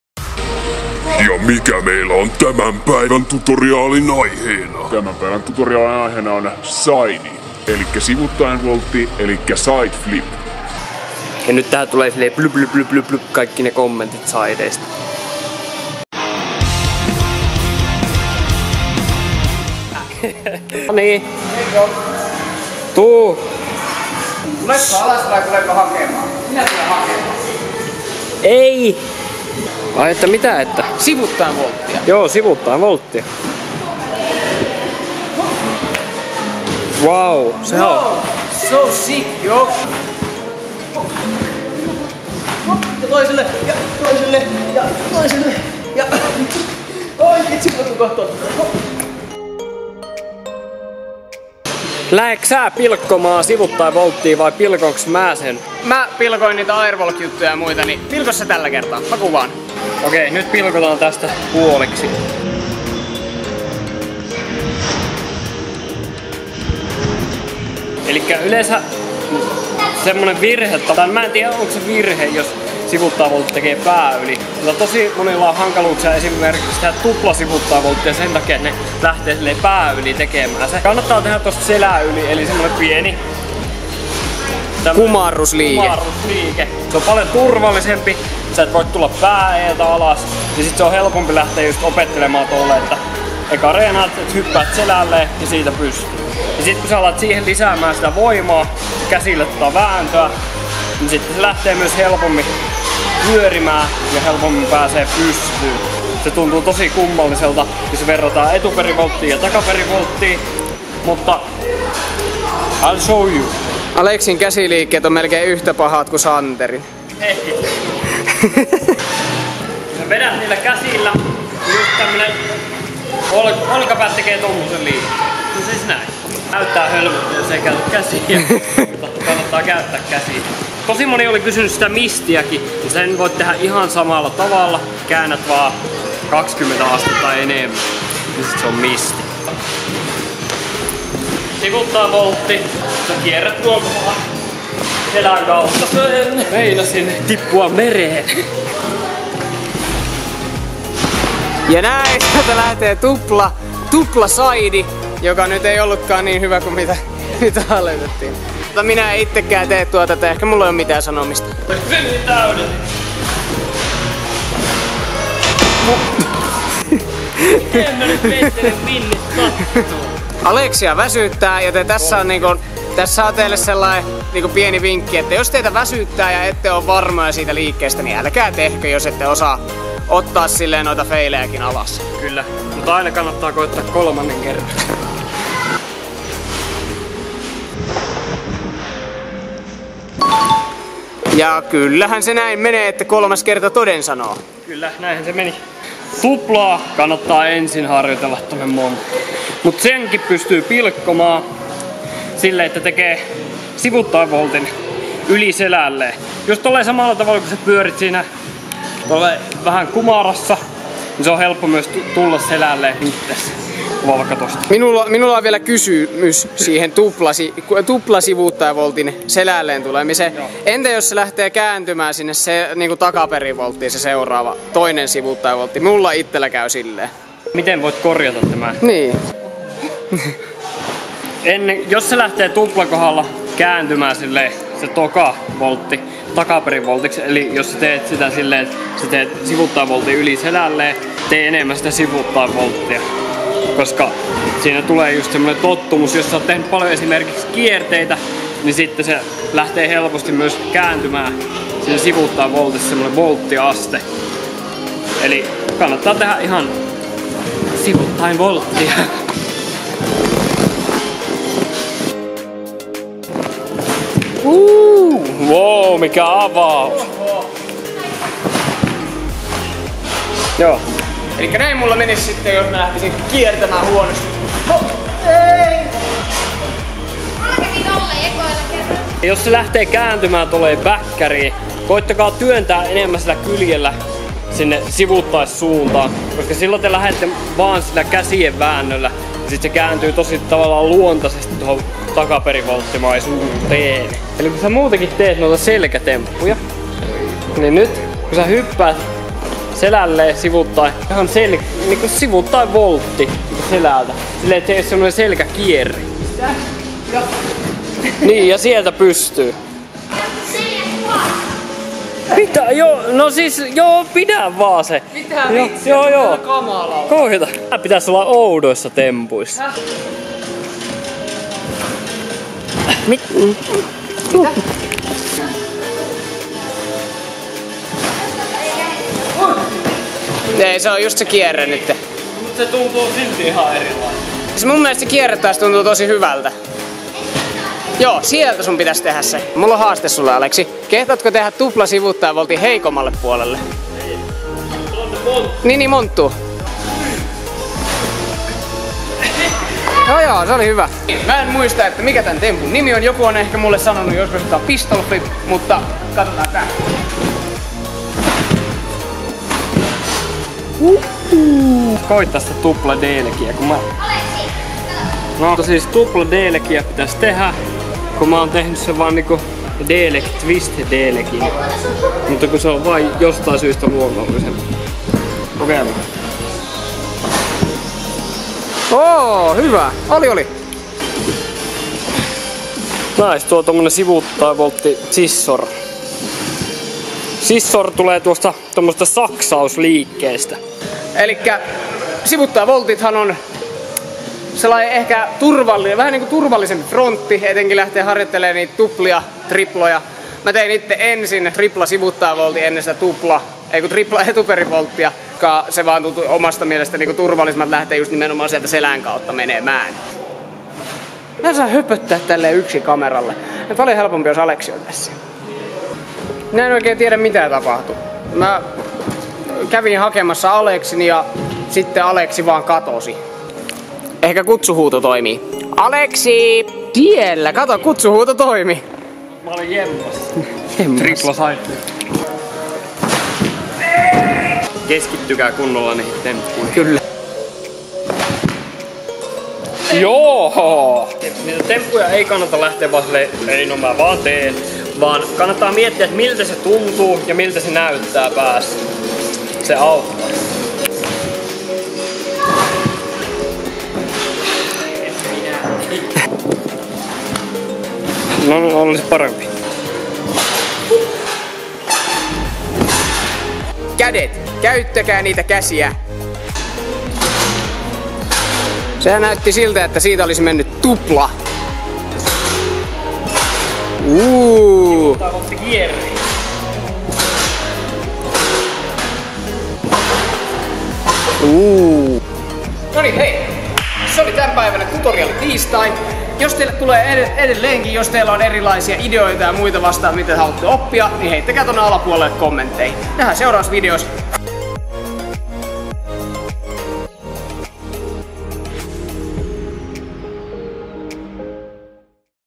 Tutoriaali Tiistai - Sideflip
Tälläkertaa Aleksi ja Jani avaavat eeppisen kysytyn sideflipin harjoittelu tekniikoita! Auttavatko nämä vinkit sinua?